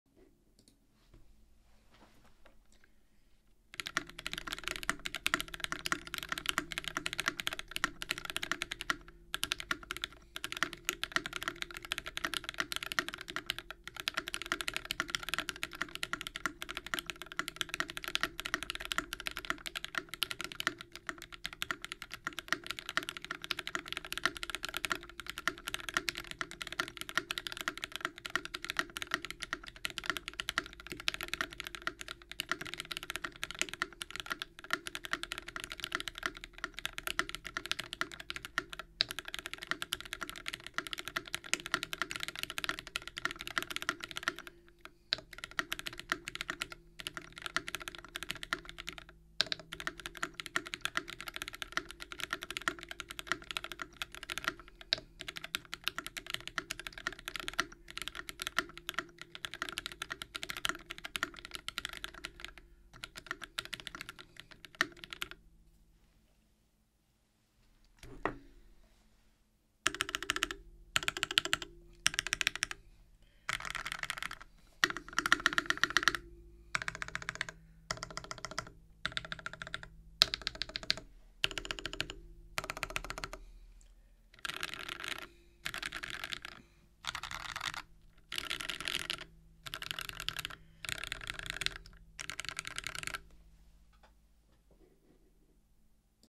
Aegis typing test
truly a board of its time and I wouldn't want it any other way

aegis is one of the boards that I truly longed for ever since getting into the hobby in 2019, and having the opportunity to purchase this one was something I couldn't pass up. gateron linears on brass are truly a product of 2018/2019 and this combination sounds really good.

fun fact:  @TaehaTypes actually built this one over three years ago during one of his earlier streams. the stabs still hold up after all this time.

specs:
- v1 Tangerines (Gateron yellow recolor, 3204)
- full brass plate
- JTK Griseann
- various Yetis